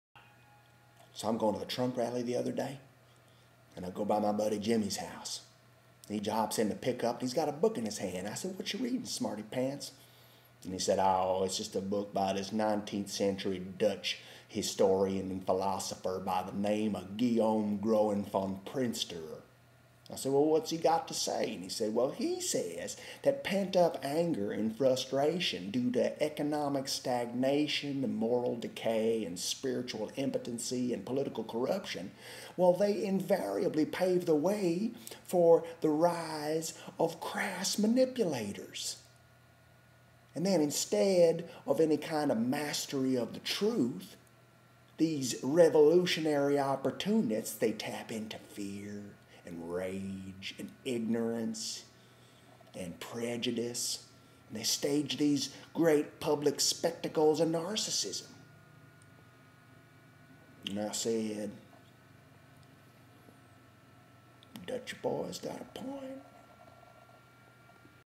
Dennis has an epiphany
Dennis picks up his friend Jimmy and Jimmy tells Dennis about a book he's been reading.